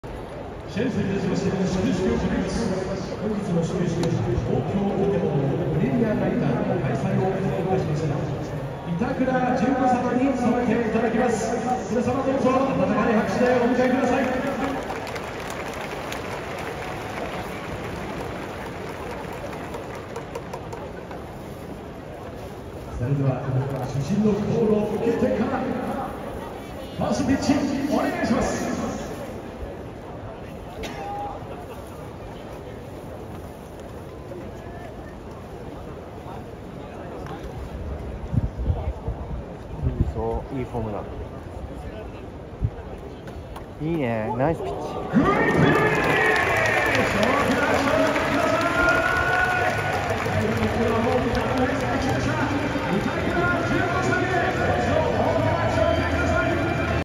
つば九郎、始球式で審判を務める 2023/6/13
2023/6/13 神宮球場
東京ヤクルトスワローズ VS 福岡ソフトバンクホークス

ナイスピッチ